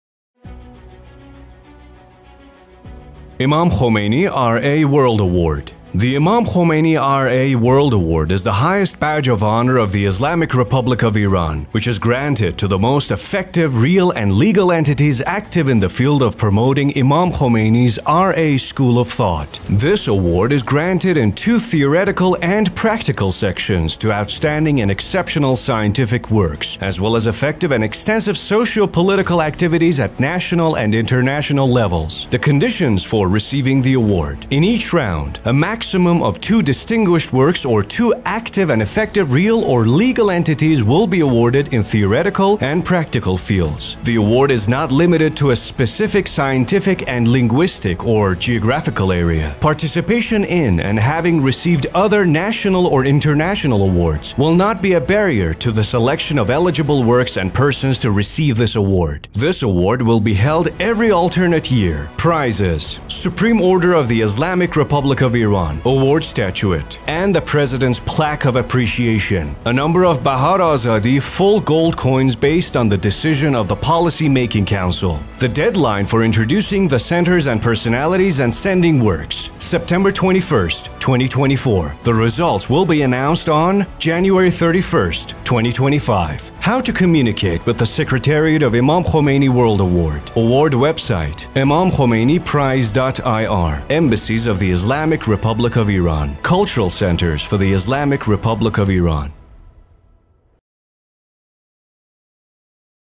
Imam Khomeini (RA) World Award
Imam Khomeini Global Award är Islamiska republiken Irans mest framstående pris, både på nationell och internationell nivå, som genomförs i syfte att identifiera och introducera framstående vetenskapliga och inflytelserika verk och intellektuella och sociopolitiska aktiviteter inspirerade av imam Khomeinis tankar och förhållningssätt.

Grundaren av Irans islamiska revolution, Imam Khomeini (fvmh), var en unik personlighet som, i en tid präglad av avancerad teknologi och århundradet av eld, ilska och förtryck, förlitade sig på islams väg och det Helige Sändebudets (Gvhf) liv och de felfria Imamerna (fvmd) och islams rena läror och grundade en revolution i Iran, vars strålar upplyste andra islamiska länder. Dagens uppvaknande och medvetenhet i den islamiska världen och muslimernas obevekliga kamp i länder som Palestina, Libanon, Irak, Afghanistan och andra delar av världen står på något sätt i skuld till imam Khomeinis (fvmh) tankar och ideal.

I syfte att hålla Imam Khomeinis (fvmh) namn och väg vid liv i världen, föreslog Islamic Culture and Relations Organisation denna utmärkelse till Kulturrevolutionens Högsta råd 2011 och gjorde några ändringar 2023. Det är för att göra en uppmaning till deltagande, och efter att ha mottagit arbetena i de teoretiska och praktiska avsnitten kommer den att introducera de utvalda verken efter att de har bedömts av en expertkommitté.

Vi finns på:
🌐 iranculture.se